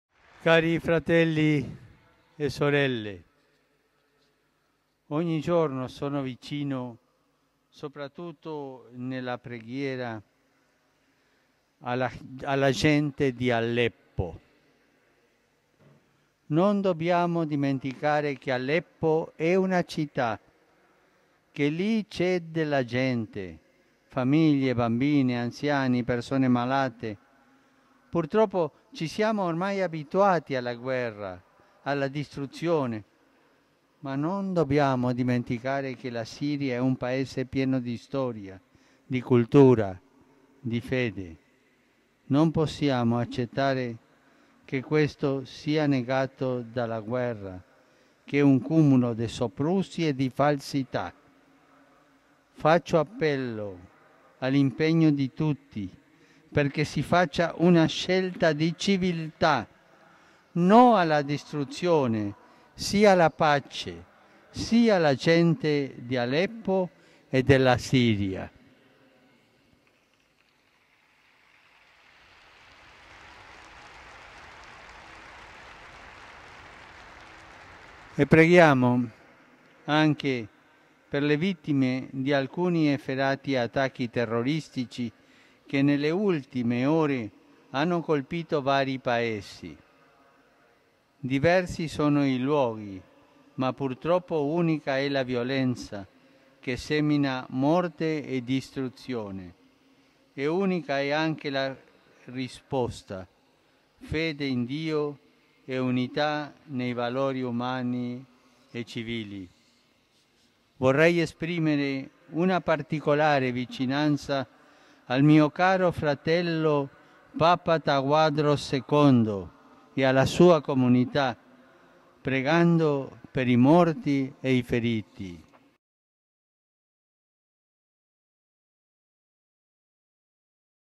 Appello Angelus 11-12-2016
Pace ad Aleppo: la guerra è un cumulo di soprusi e falsità

Alla fine dell’Angelus, il Pontefice ha lanciato un nuovo appello per la pace in Siria e ha pregato per le vittime degli attentati che nelle ultime ore hanno colpito diversi Paesi.